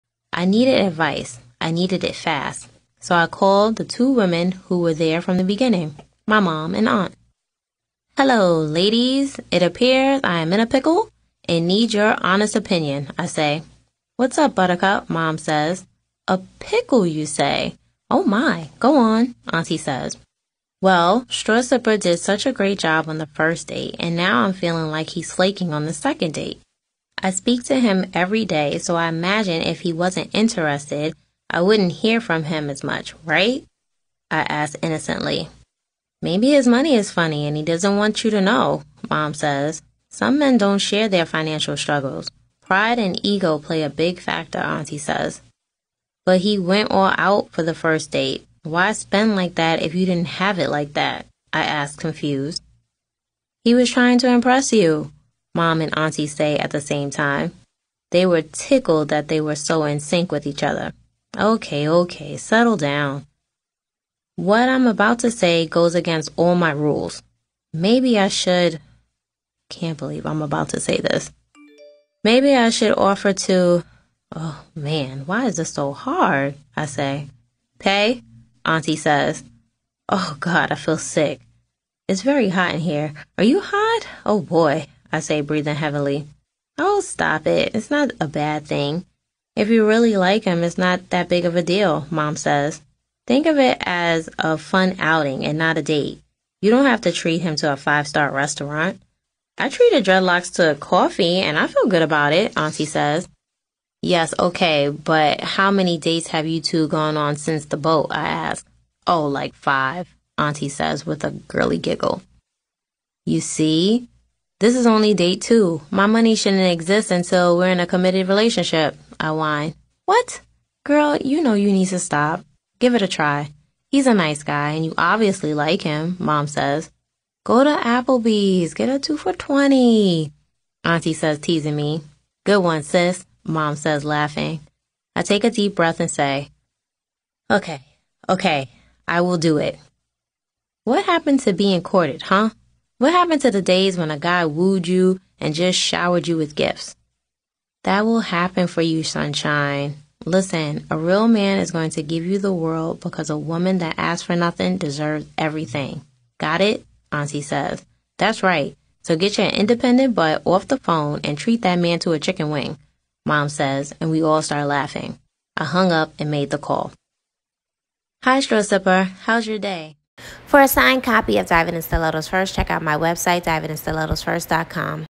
Story Time: Chapter 9: The Hookup...My date with StrawSipper
Have you ever been wondering what is taking  so long for a second date? I mean after all they told you how much they like you right? Instead of overthinking it alone I decided to send in the big troops. Who do you call on when you need solid advice? I am blessed to have a Mom and Aunt who not only listen but can’t wait to share their wisdom. 🙋🏾‍♀️
💚Here’s a scene from my Book  Diving in Stilettos First:  Memoirs of Dating Mr. Right Now📚
After my annulment I dived back into the dating pool of NYC in search of love again. Here I’m considering asking  a guy out for the first time. Of course I would have preferred he asked but I needed a little pep talk to assure me I was doing the right thing. 👀
💚So here’s one of many dating stories. Let me know if you can relate.
🧐Have you ever asked someone out on a date? How’d it go? Who Paid?🤷🏾‍♀️